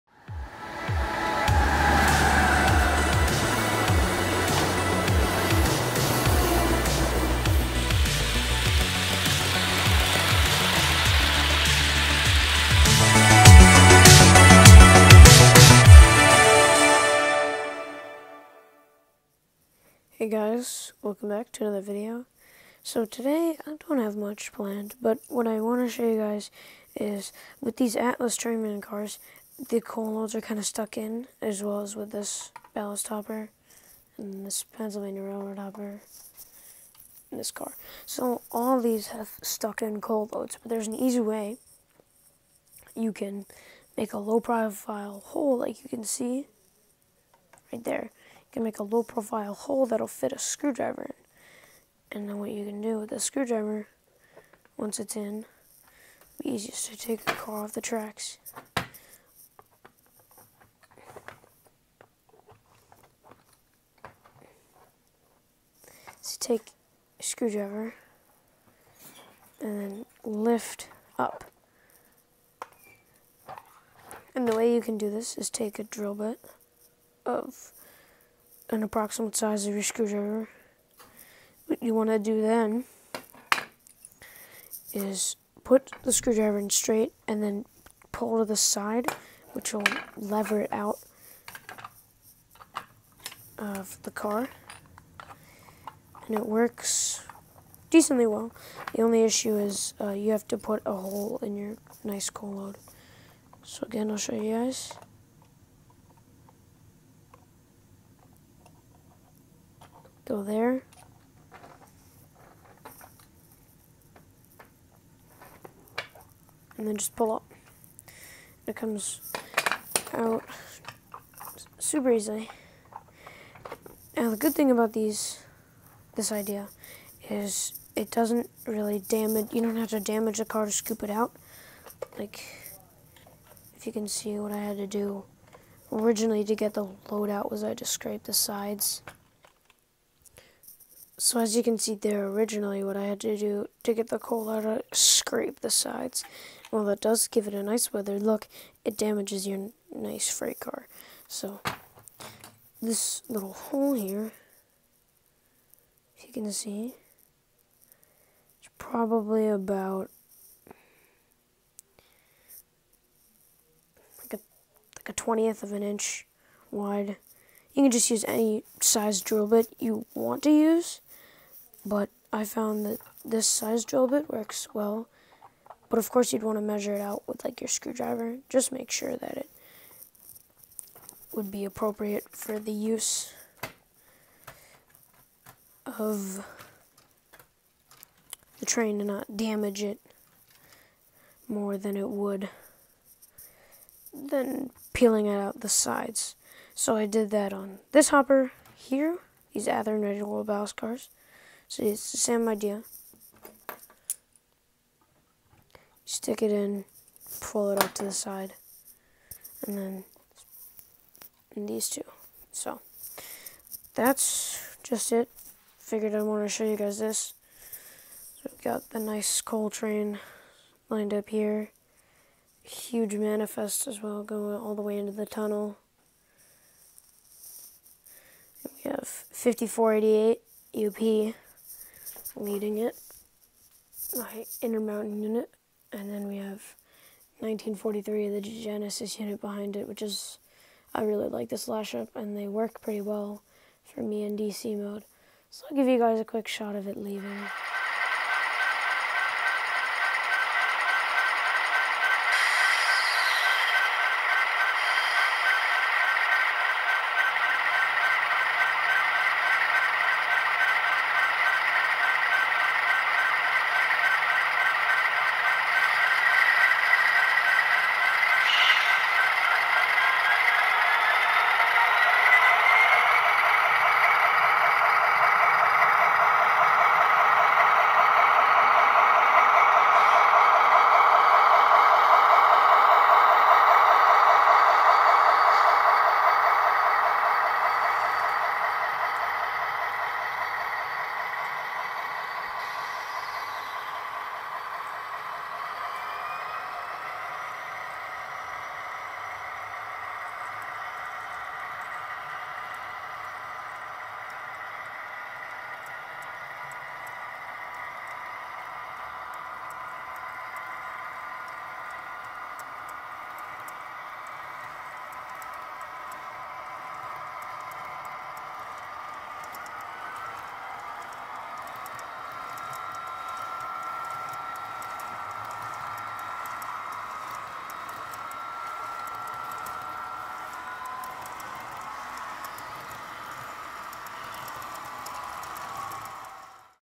My coal car unloading technique
Here’s a video on an easy way to remove your coal loads!